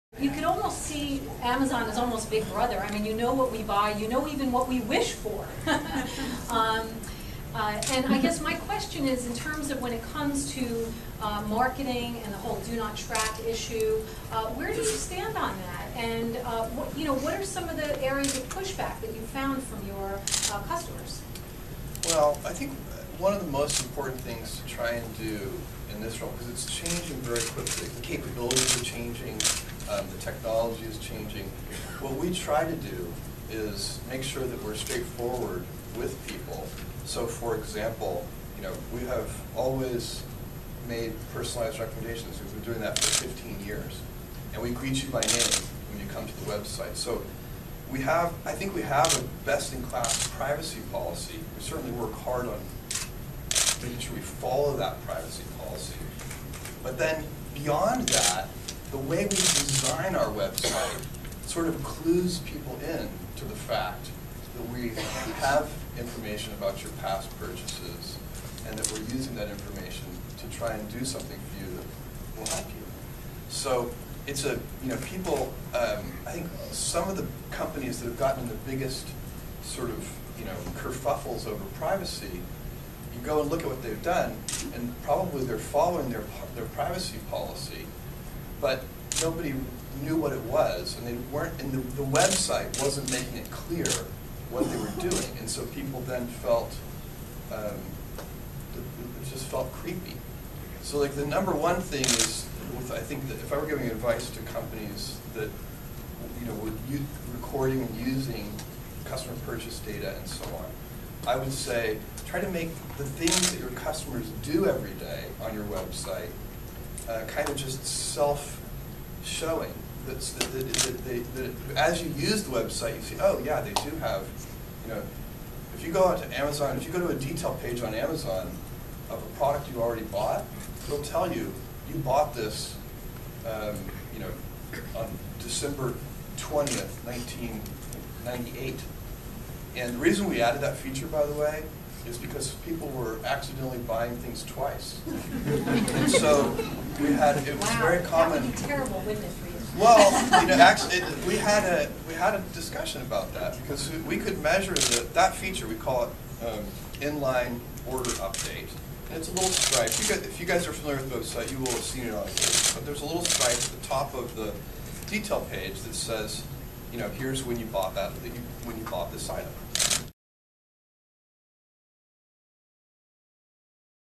Jeff Bezos of Amazon on privacy and tracking purchases
Jeff Bezos, CEO of Amazon talks about his company tracking customers purchasing habits and privacy issues at a ShopSmart magazine event organized by Consumer Reports